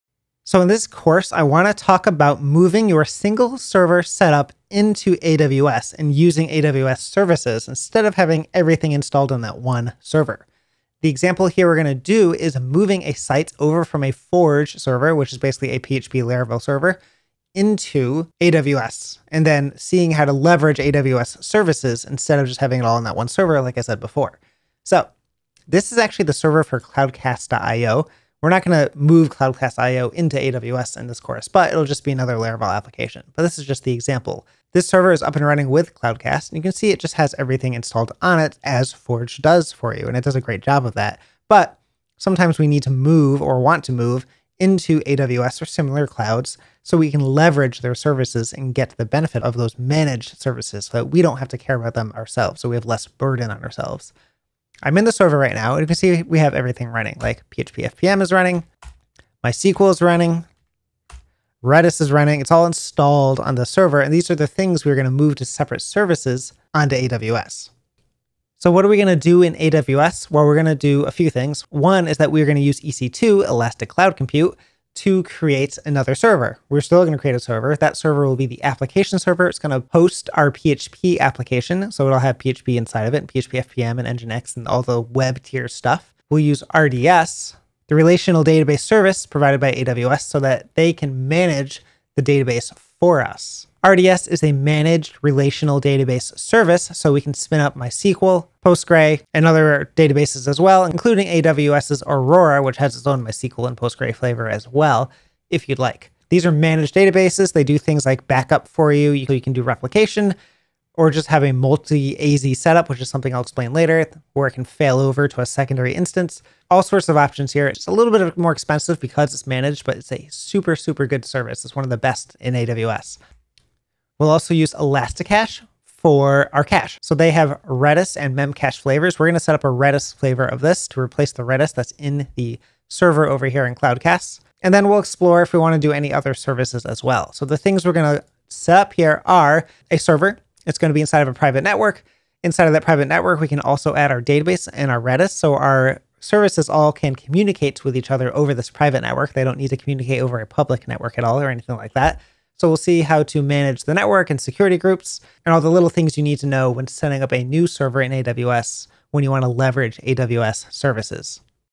Onboarding AWS - Getting started with a server, RDS database, and ElastiCache instance.
We cover what this Onboarding AWS course will teach:

1. Creating EC2 servers
2. Making a re-usable server image
3. Creating and monitoring an RDS MySQL instance
4. Creating and monitoring a ElastiCache Redis instance

Of course there's more to it than that! We cover a LOT of little details that are confusing to newcomers to AWS.